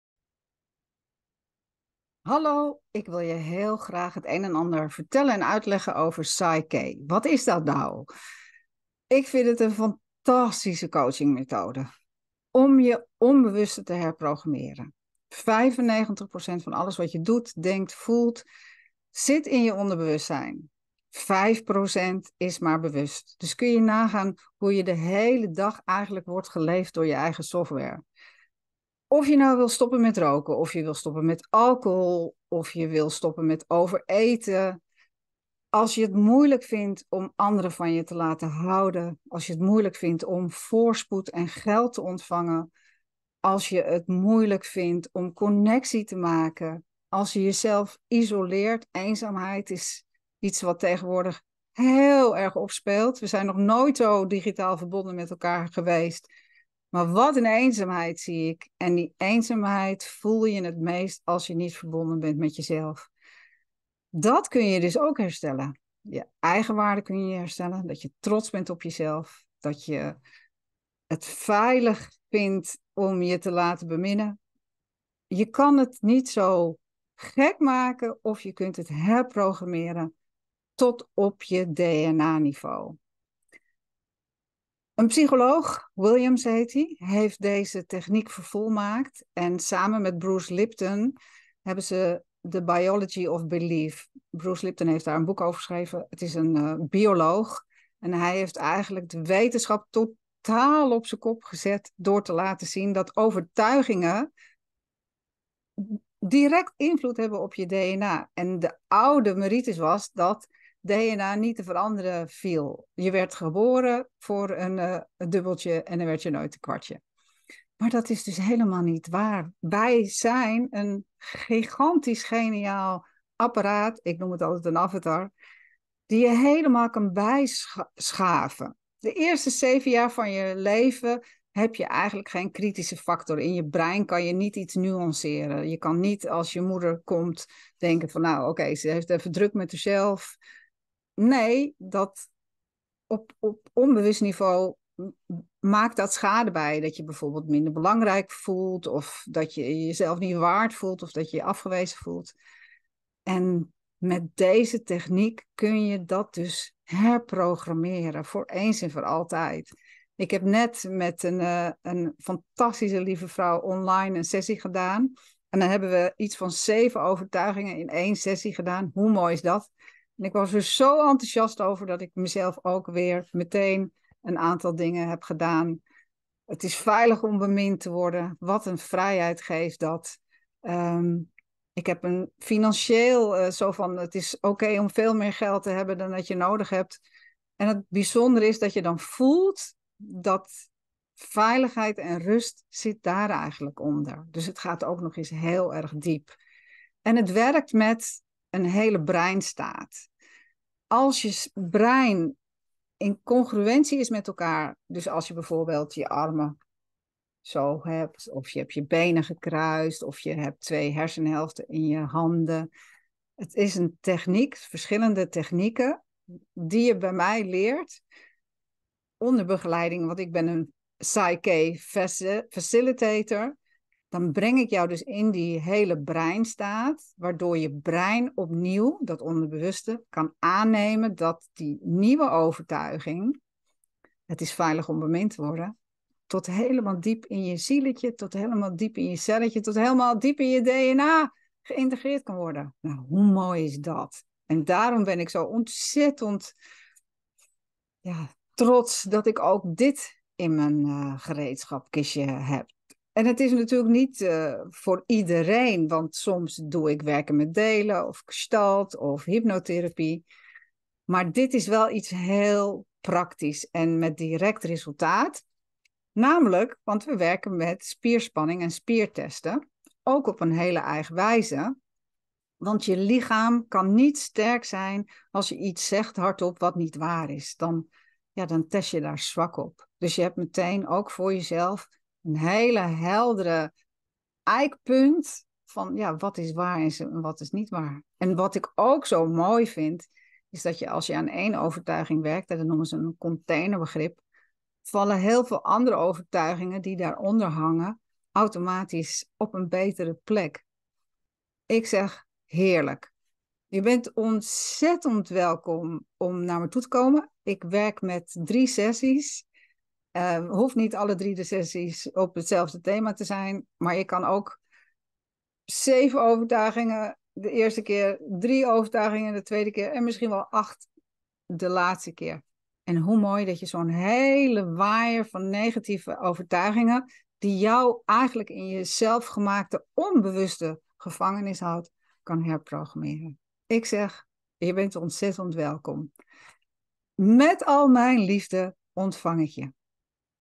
Afl. 50 Reken voorgoed af met je demonen! Verslavingen, gebrek aan liefde & eenzaamheid PSYCH-K
Wil jij voorgoed afrekenen met je demonen? Met PSYCH-K kun je in drie sessies van je alcohol- of eetverslaving en roken afkomen. Je hebt echt 3 sessies nodig voor uitzonderlijk resultaat. In al m'n wetenschappelijke consulten, gebruik ik m'n lijntje naar boven!